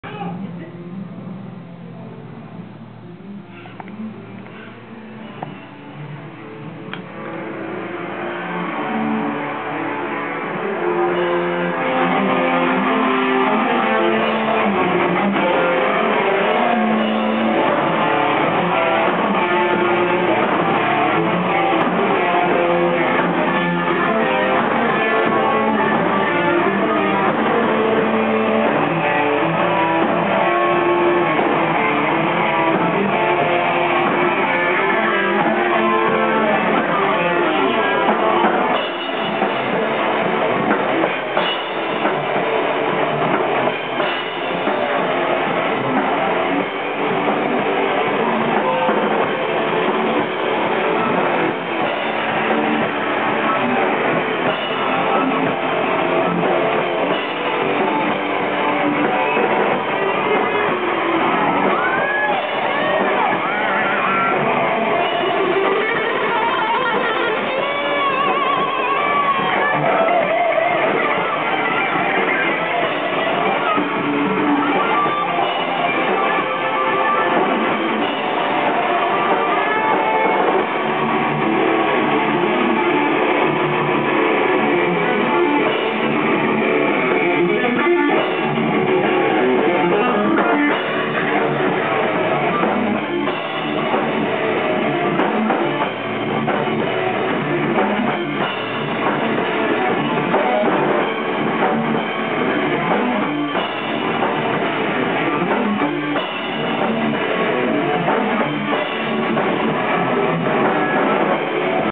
Lithium's First Practice
This is their first practice and first original song.